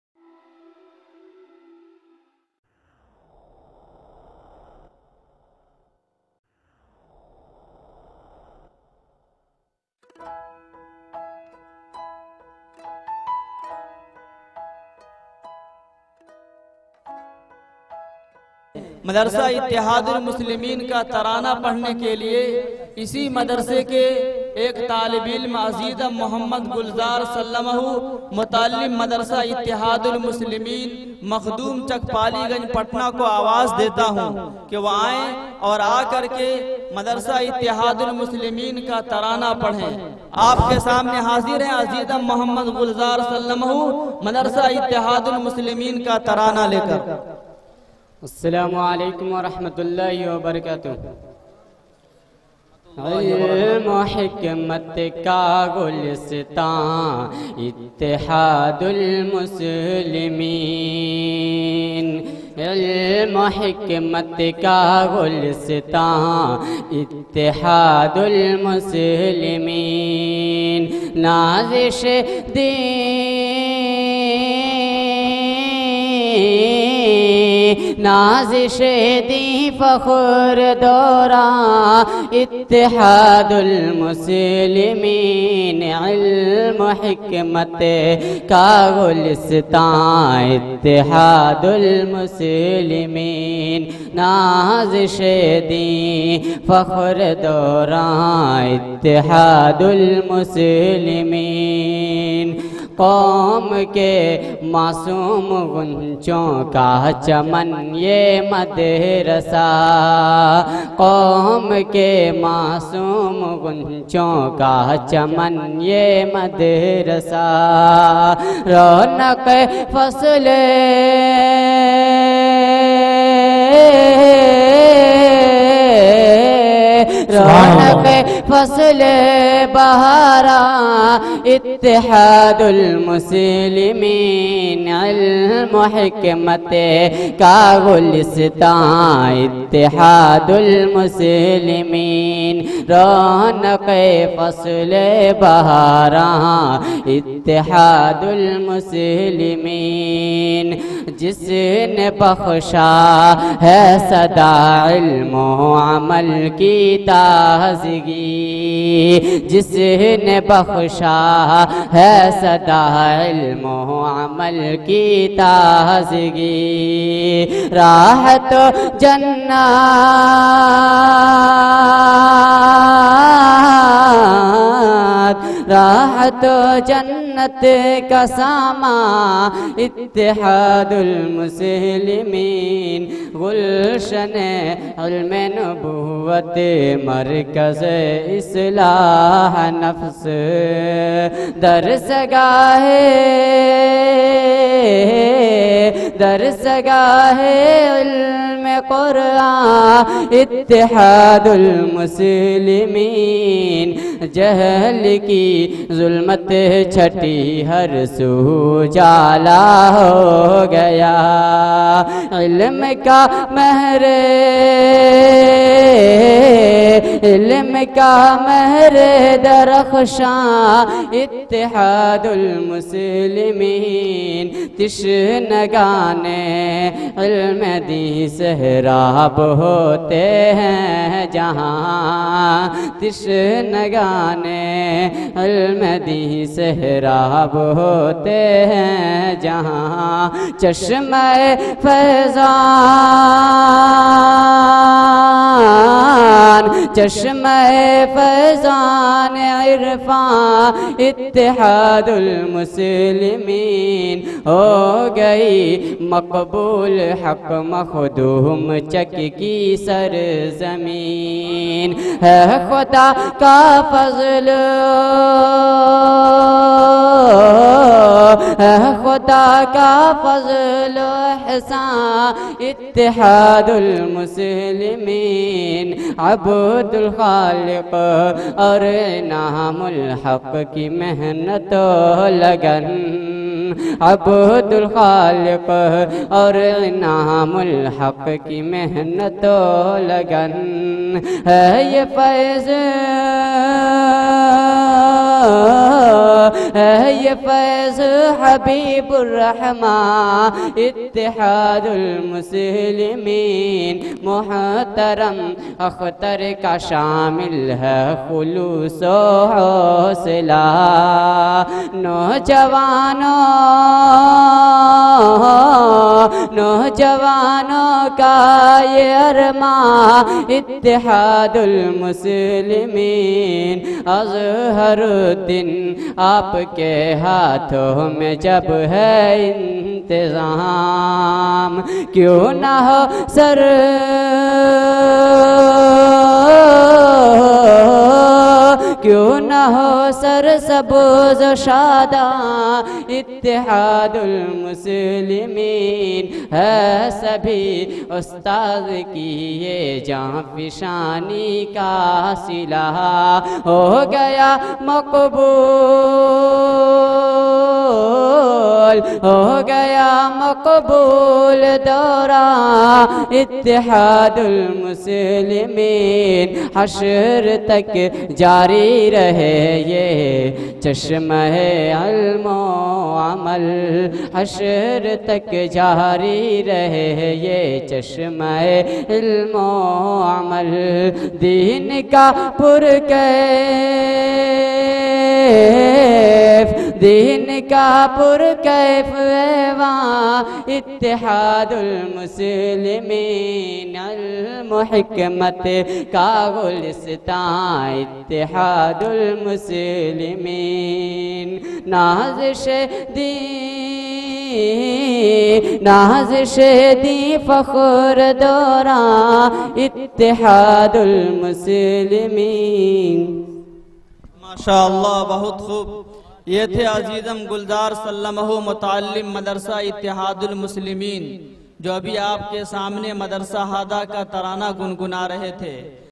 Tarana Md Gulzar Jalsa Dastar Bandi Makhdum Chak Patna 2019 JK Mushaira Media
Tarana Md Gulzar
Jalsa Dastar Bandi Makhdum Chak Pali Ganj Patna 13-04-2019
Jalsa e Sadarat: Mufti Mohd. Anzar Sb Qasmi
Nazim e Jalsa: Mufti Abdur Rahman Qasmi
Nazim Madrasa Md. Azharuddin Nomani
Sadre Madrasa: Md Anamul haque
Madrasa Secretary: Md. Abdul Khalik
Managed By: Madrasa Ittihadul Muslimeen Makhdum Chak
Venue: Makhdum Chak Pali Ganj Patna Bihar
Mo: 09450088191